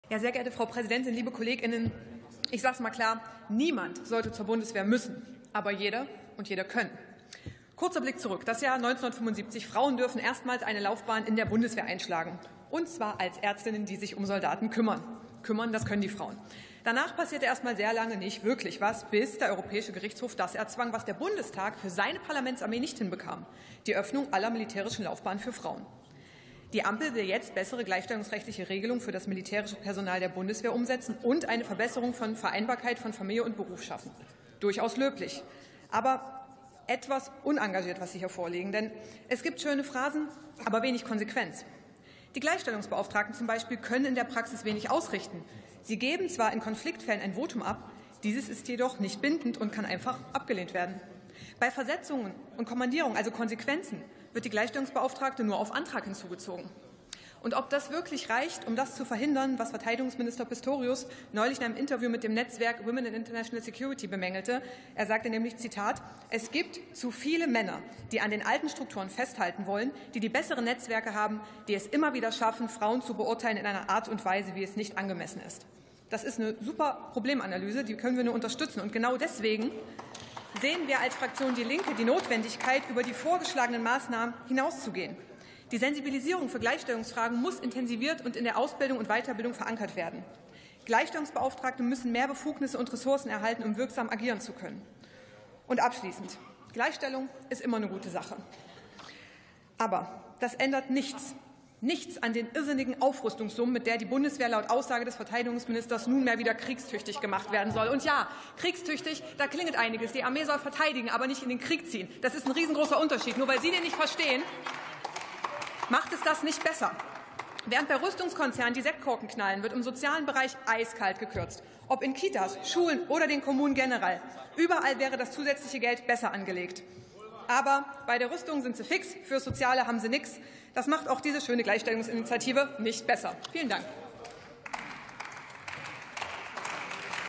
Gleichstellung in der Bundeswehr?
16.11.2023 - Heidi Reichinnek: Soldatinnen und Soldaten sollen gleichgestellt werden. Der Vorschlag der Regierung sieht aber nicht vor, Gleichstellungsbeauftragte mit ausreichenden Befugnissen auszustatten. Dafür heißt es jetzt "Frau Offizierin" - mal wieder gut gemeint, aber nicht gut gemacht.